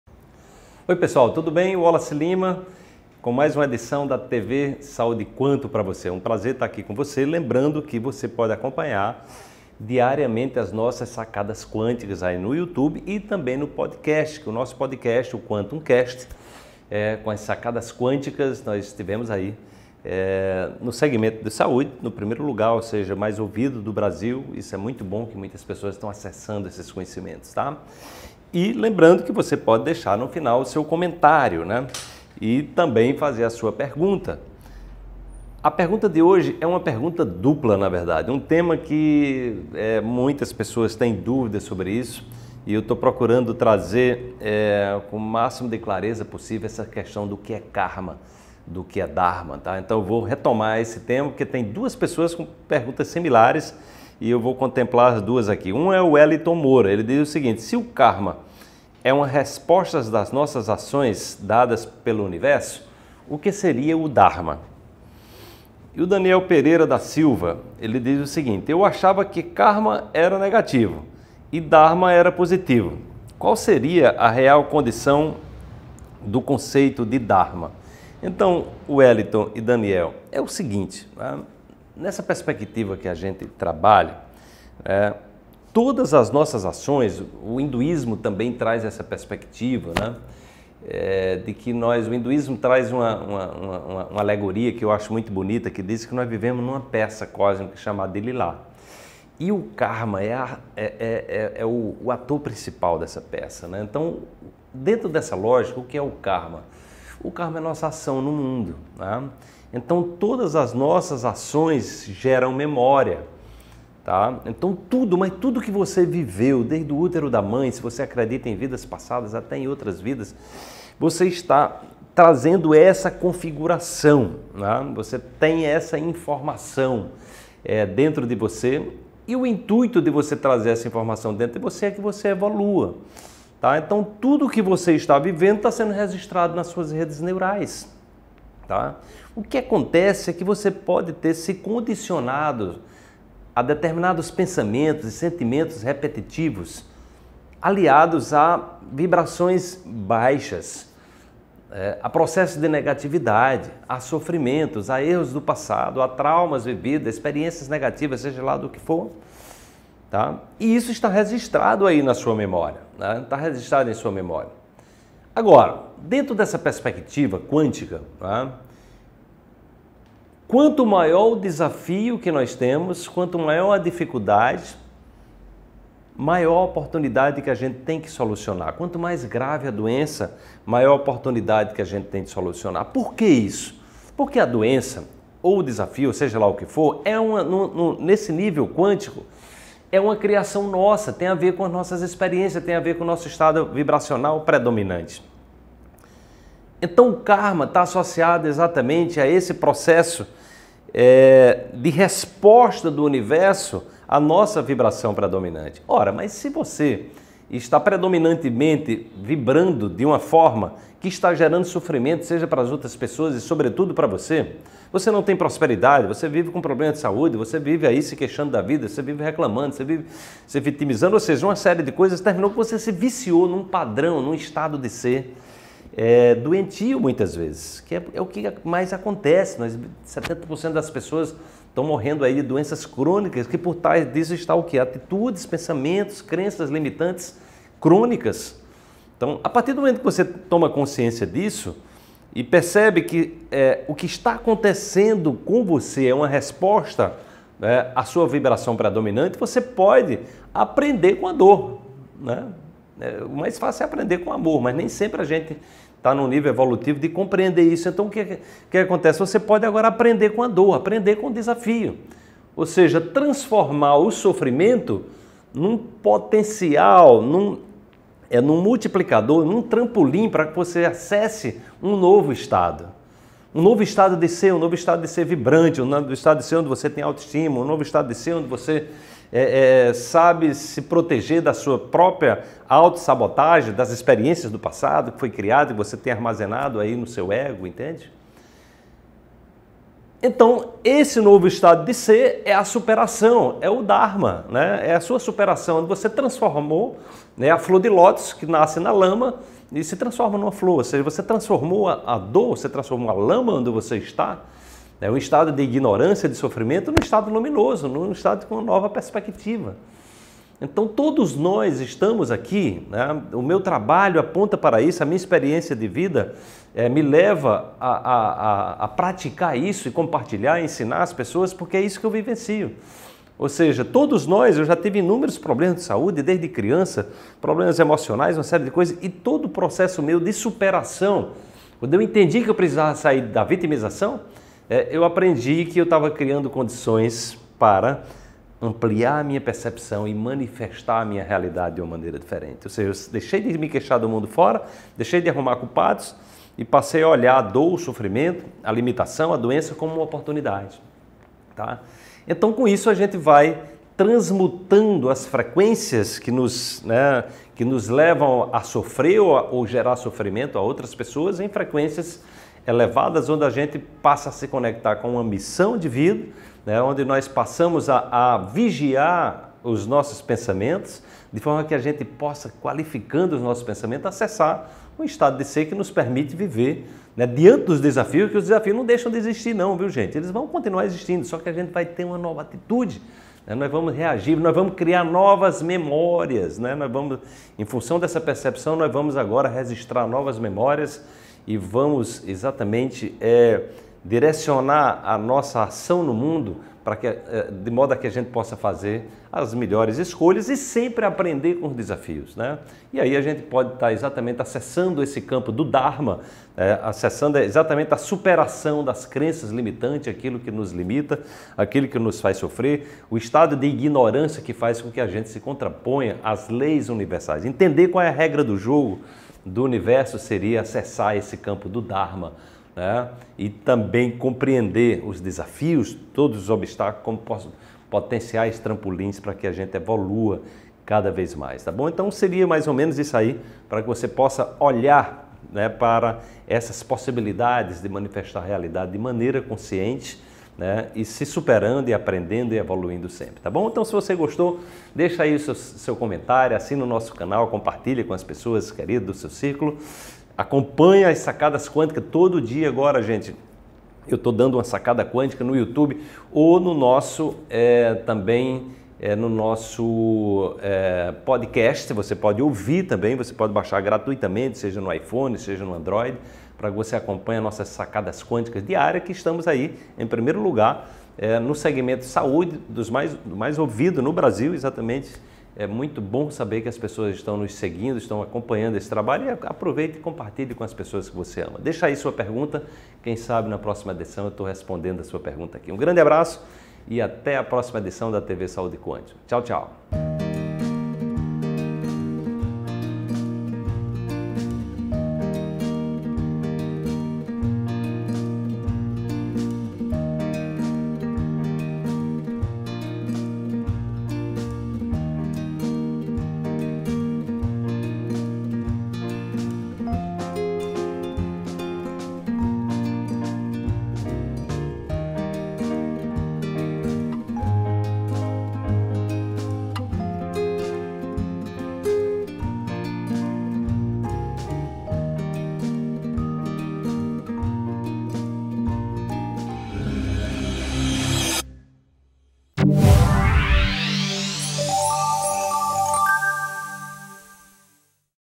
TV Saúde Quantum 042: Saiba a Diferença Entre Karma e Dharma | Wallace Liimaa
✭✭✭✭✭ Saiba a Diferença Entre Karma e Dharma. Nesse vídeo apresento uma reflexão sobre as nossas ações e as respostas do universo. Assista o vídeo e comprove!